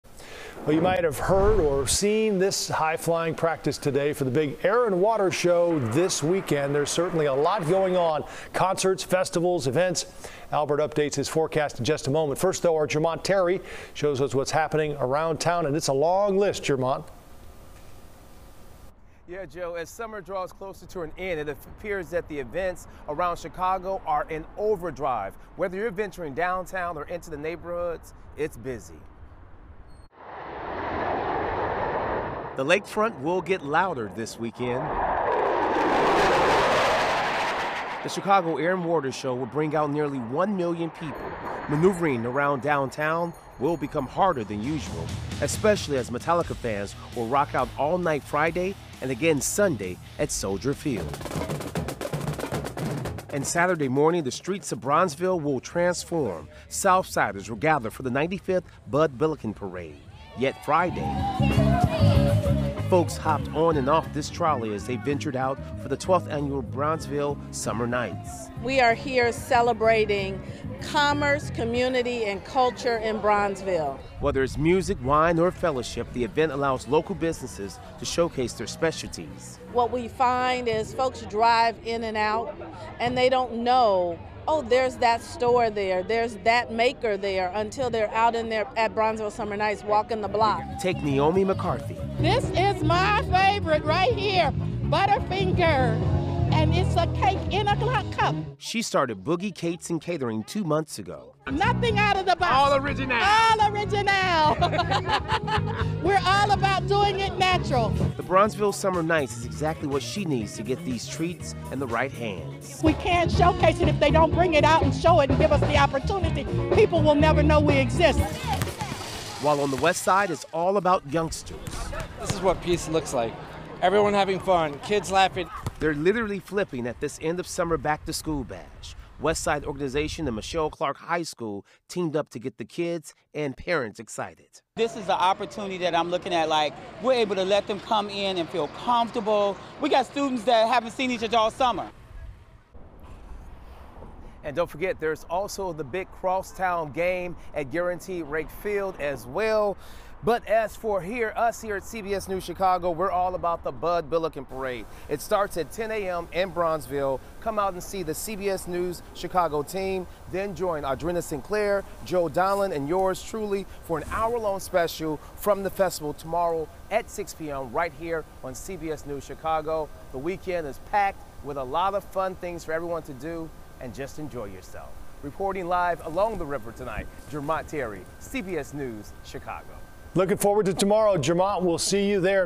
Air & Water Show, Bud Billiken Parade highlight busy Chicago weekend
Those in Chicago late this week might have heard the high-flying practicing for the big Air and Water Show this weekend. And that's just one of the big events this weekend which will be packed full of concerts, festivals and other happenings.